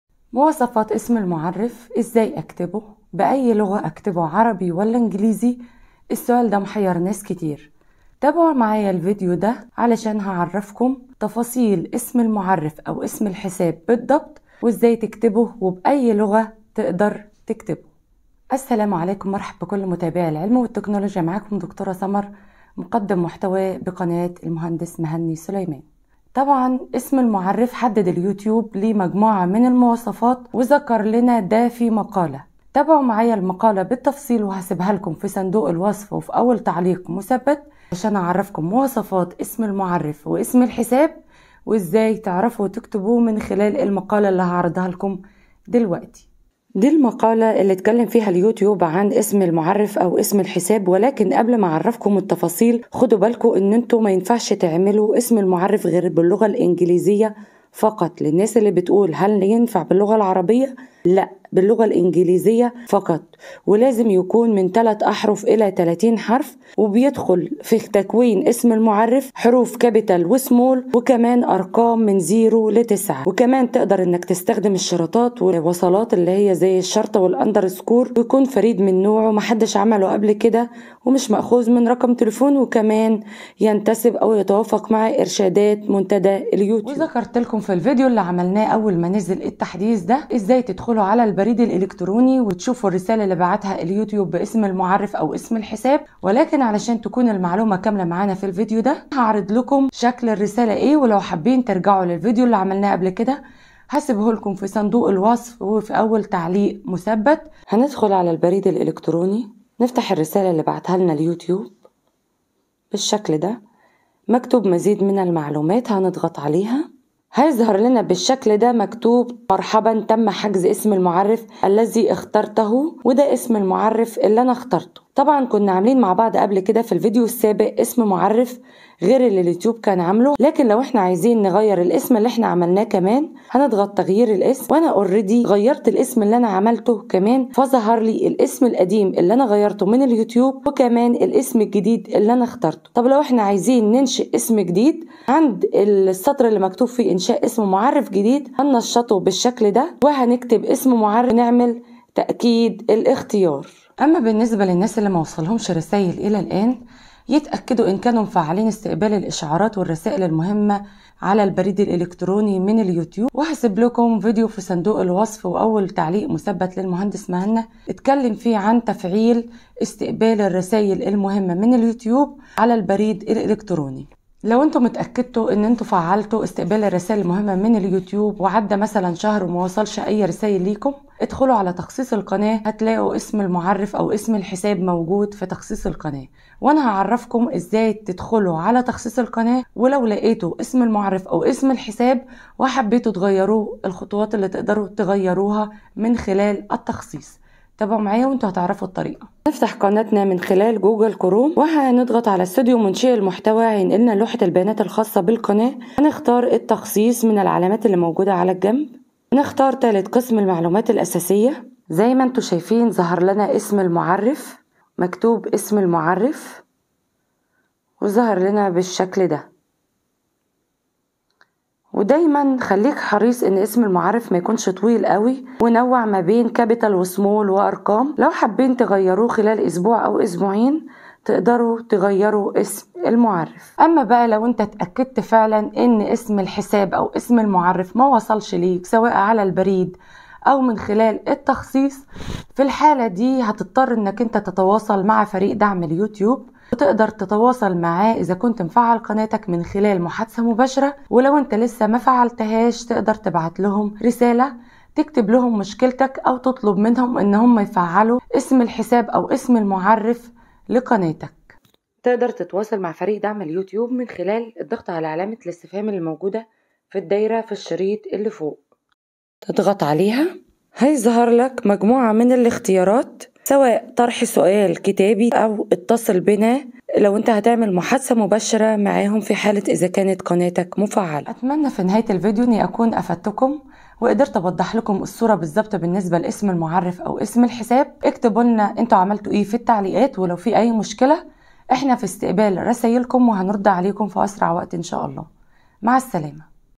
توضيح هام👍💯 تفاصيل لغة اسم المعرف( اسم الحساب ) لقناتك على اليوتيوب🤑 الربح من اليوتيوب2023
توضيح تفاصيل اسم المعرف( اسم الحساب ) لقناتك على اليوتيوب الربح من اليوتيوب2023
اسم المعرف لقناتك على يوتيوب، لغة اسم المعرف او اسم الحساب لقناتك على يوتيوب التحديثات الجديدة من اليوتيوب والعثور على قناتك بسهولة | أسماء الحسابات او اسم المعرِّف
تحديثات جديدة لليوتيوب 2023 لكل الحسابات والقنوات اسم مستخدم او اسم معرف خاص بكل قناة سيساعد منشئ المحتوي على ظهور قناته في محركات البحث وزيادة عملة التواصل عبر منصة اليوتيوب
نقدّم لك أسماء الحسابات في YouTube‏ ونوضح فائدة اسم المعرف لقناة اليوتيوب
ارسل يوتيوب رسالة على البريد الالكتروني باسم المعرف الذي تم تحديده لكل قناة.
أصبح بإمكانك الآن اختيار اسم حساب لقناتك.
 إذا كان لديك عنوان URL مخصّص لقناتك، فإننا نحجز لك هذا العنوان ليكون اسم حسابك في معظم الحالات. إذا أعجبك الاسم يمكنك الاحتفاظ به، أو يمكنك تغييره إذا أردت اسم حساب مختلفًا. إذا لم يكن لديك عنوان URL مخصّص في الوقت الحالي، فسيكون بإمكانك أيضًا اختيار اسم حساب. إنّ أسماء الحسابات فريدة، لذلك في حال اختيار واحد منها، لن يتمكّن مستخدم آخر من اختياره.
إرشادات اختيار الأسماء المعرِّفة
وسائل التواصل
قناتنا الجديدة على التليجرام
قناتنا الثانية على اليوتيوب
Youtube.com/Mohanysoliman2